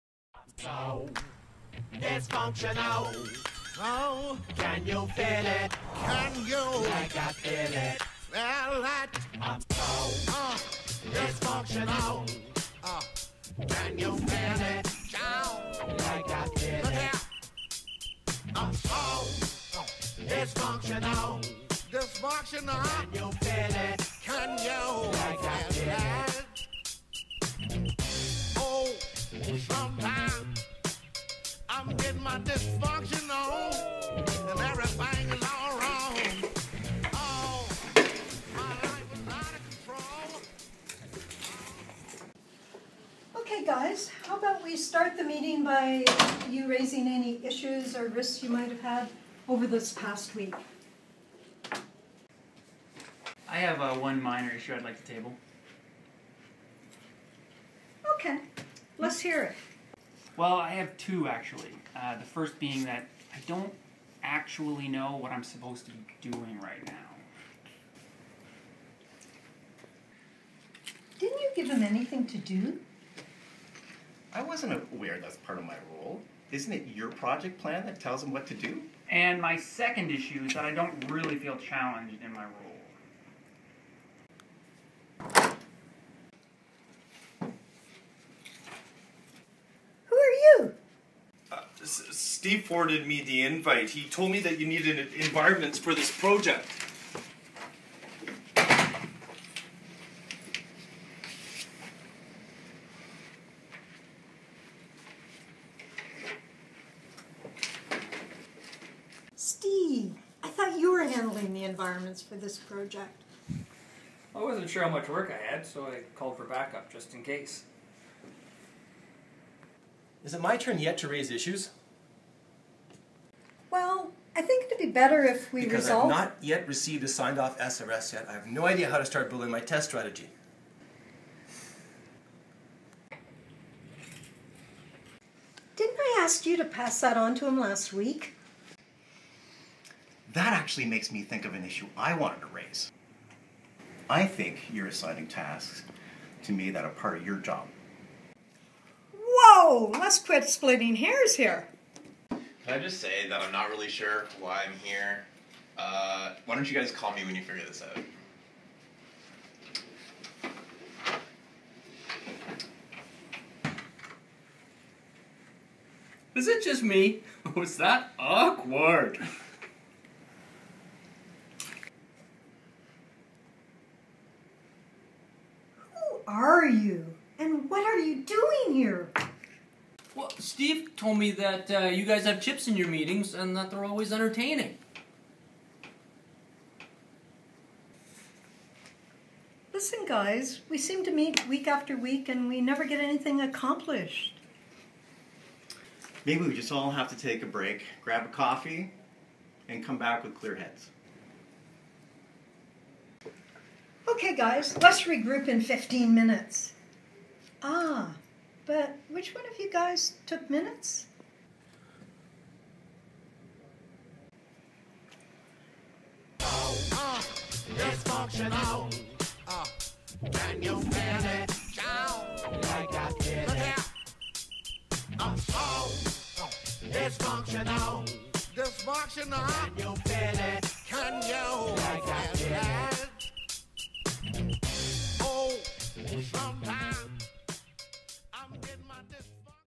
The Dysfunctional Team May 2014
A humorous look at a dysfunctional team meeting. Video is meant to highlight the many mistakes made which reduce meeting productivity and increase frustration.